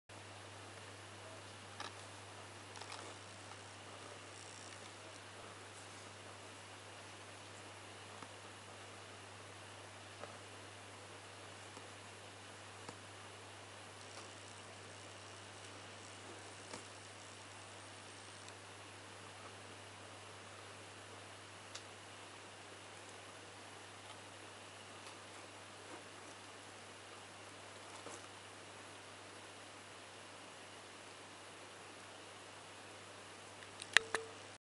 Pancake tortoise (Malacochersus tornieri)
1-month old Pancake tortoise (Malacochersus tornieri) feeding on a dandelion flower.